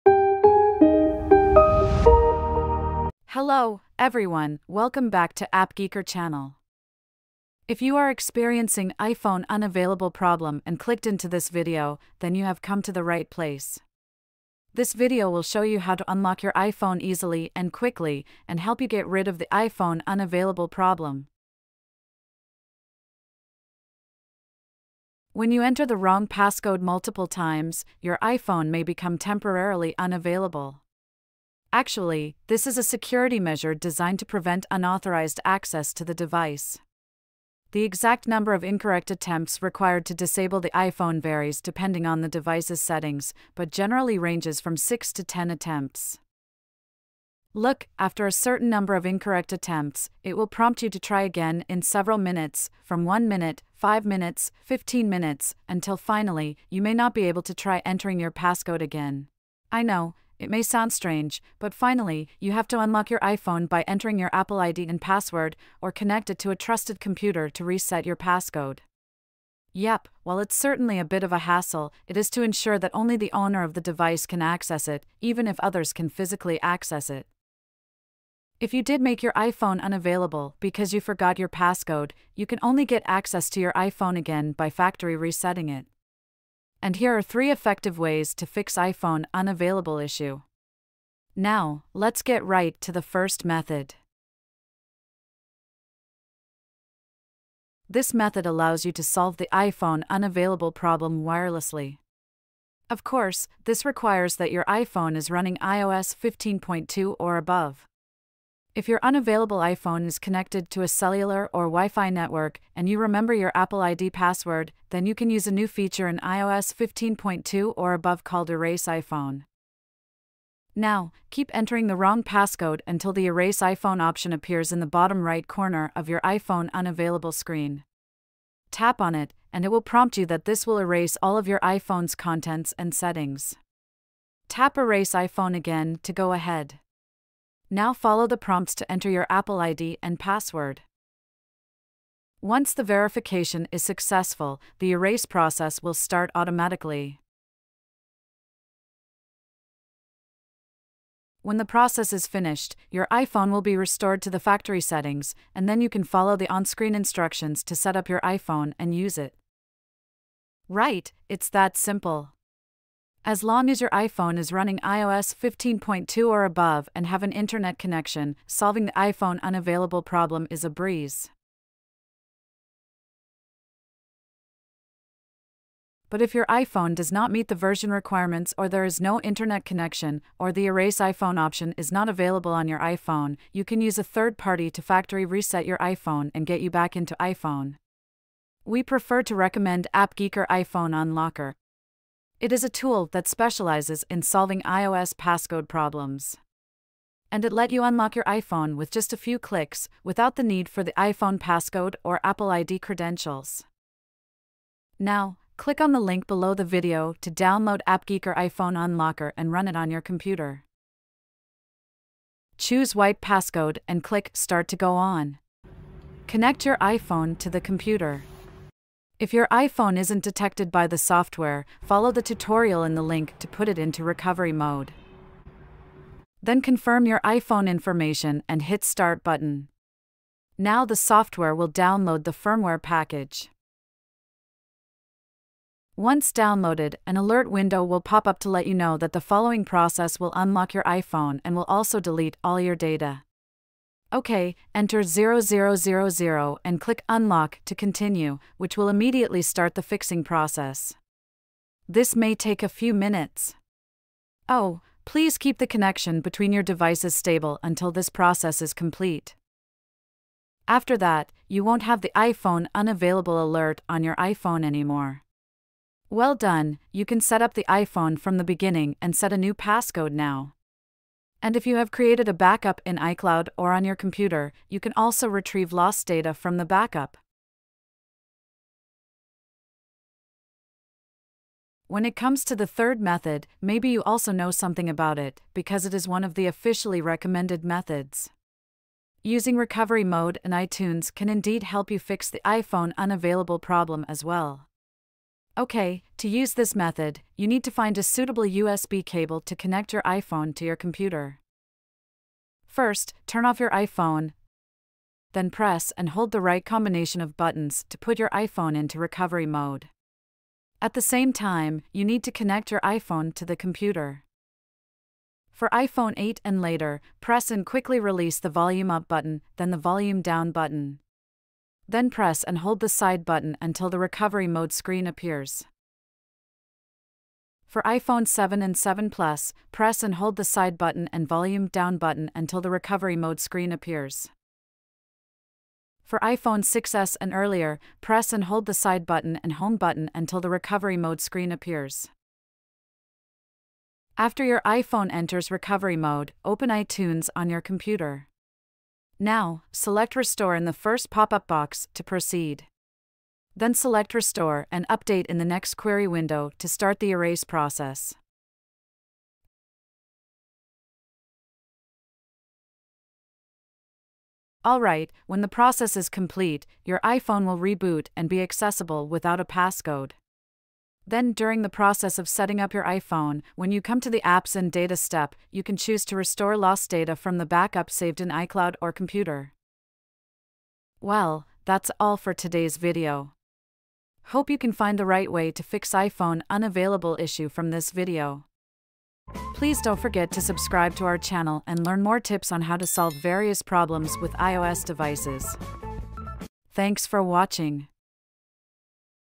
iPhone Unavailable? Why and How to Fix It!
If you get an iPhone Unavailable alert on your iPhone screen, whether it's because of forgotten passcode or entered wrong passcode too many times, don't worry, this video will show you how to fix this tricky problem

00:03 Intro
00:22 Explanation of iPhone Unavailable
01:37 Erase iPhone
03:08 iPhone Unlocker
04:41 iTunes

Attention:
Resetting your iPhone to factory settings without a password will delete all the data and settings stored in your iPhone. But if you have a backup in iCloud or computer then you can restore your data from iCloud or computer after the reset process is over.